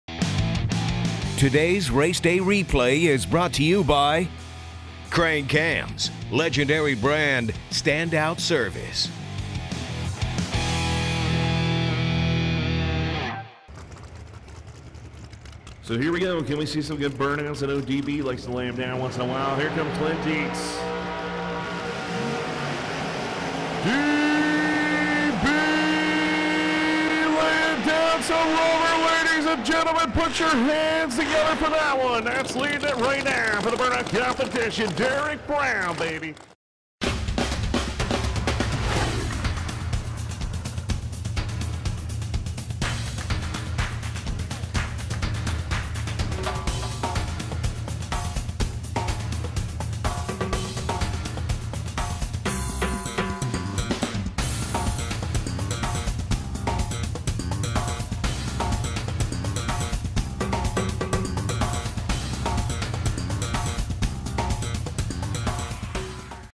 Derrick Brown lays down a huge burnout Top Sportsman
Top Sportsman’s Derrick Brown lays down a massive burnout for the burnout contest in the PDRA World Finals